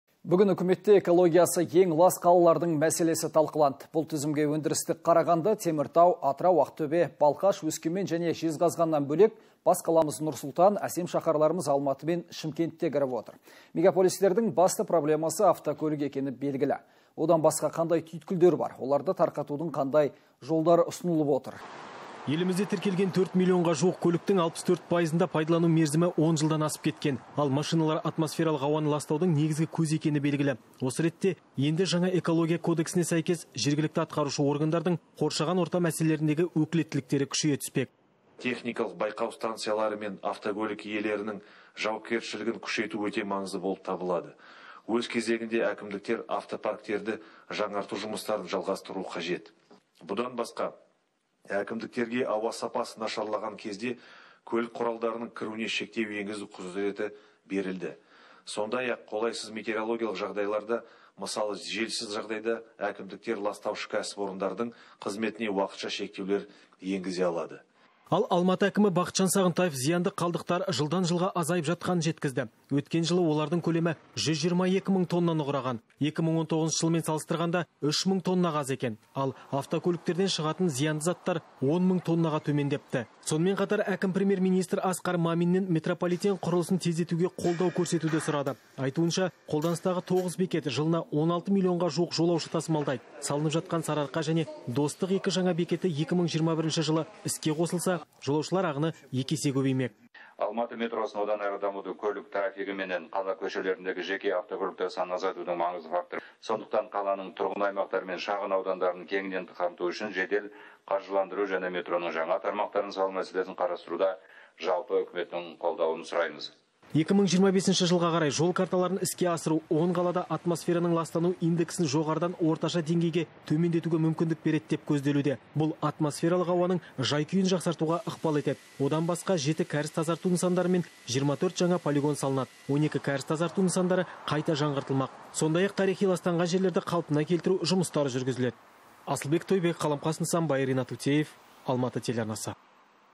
Таза ауа – жанға дауа: Үкіметте экологиясы ең лас қалалардың мәселесі талқыланды (15.06.21)
Үкіметте экологиясы ең лас қалалардың мәселесі талқыланды. Бұл тізімге өндірістік Қарағанды, Теміртау, Атырау, Ақтөбе, Балқаш, Өскемен және Жезқазғаннан бөлек бас қаламыз Нұр-Сұлтан, әсем шаһарларымыз Алматы мен Шымкент те кіріп отыр. Мегаполистердің басты проблемасы автокөлік екені белгілі, одан басқа қандай түйткілдер бар? Оларды тарқатудың қандай жолдары ұсынылып отыр? Астаналық Almaty.tv тілшісі Қаламқас Нысанбай тарқатсын.

Елімізде тіркелген 4 миллионға жуық көліктің 64 пайызында пайдалану мерзімі 10 жылдан асып кеткен. Ал, көліктер  атмосфералық ауаны ластаудың негізгі көзі екені белгілі. Осы ретте, енді жаңа экология кодексіне сәйкес жергілікті атқарушы органдардың қоршаған орта мәселелеріндегі өкілеттіктері күшейе түспек.

«Техникалық байқау станциялары мен автокөлік иелерінің жауапкершілігін күшейту өте маңызды. Өз кезегінде әкімдіктер автопарктерді жаңарту жұмыстарын жалғастыруы қажет. Бұдан басқа, әкімдіктерге ауа сапасы нашарлаған кезде көлік құралдарының кіруіне шектеу енгізу құзыреті берілді. Сондай-ақ, қолайсыз метеорологиялық жағдайларда, мысалы, желсіз жағдайда, әкімдіктер ластаушы кәсіпорындардың қызметіне уақытша шектеулер енгізе алады», – деді ҚР Экология, геология және табиғи ресурстар министрі Мағзұм Мырзағалиев.

Ал, Алматы әкімі Бақытжан Сағынтаев зиянды қалдықтар жылдан жылға азайып жатқанын жеткізді. Өткен жылы олардың көлемі 122 мың тоннаны құраған. 2019 жылмен салыстырғанда 3 мың тоннаға аз екен. Ал, автокөліктерден шығатын зиянды заттар 10 мың тоннаға төмендепті. Сонымен қатар, әкім Премьер-министр Асқар Маминнен метрополитен құрылысын тездетуге қолдау көрсетуді сұрады. Айтуынша, қолданыстағы тоғыз бекет жылына 16 миллионға жуық жолаушы тасымалдайды. Салынып жатқан «Сарыарқа» және «Достық» екі жаңа бекеті 2021 жылы іске қосылса, жолаушылар ағыны екі есеге көбеймек

«Алматы метросын одан әрі дамыту – көлік трафигі мен қала көшелерінде жеке автокөліктер санын азайтудың маңызды факторы. Сондықтан қаланың тұрғын аймақтары мен шағын аудандарын кеңінен қамту үшін жедел қаржыландыру және метроның жаңа тармақтарын салуды қарастыруда үкіметтің қолдауын сұраймыз», – деді Алматы қаласының әкімі Бақытжан Сағынтаев.

2025 жылға қарай Жол карталарын іске асыру 10 қалада атмосфераның ластану индексін жоғарыдан орташа деңгейге төмендетуге мүмкіндік береді деп көзделуде. Бұл – атмосфералық ауаның жай-күйін жақсартуға ықпал етеді. Одан басқа, 7 кәріз тазарту нысандары мен 24 жаңа полигон салынады. 12 кәріз тазарту нысандары қайта жаңғыртылмақ. Сондай-ақ тарихи ластанған жерлерді қалпына келтіру және жою жұмыстары жүргізіледі.

Қаламқас Нысанбай, Ринат Утеев, «Алматы» арнасы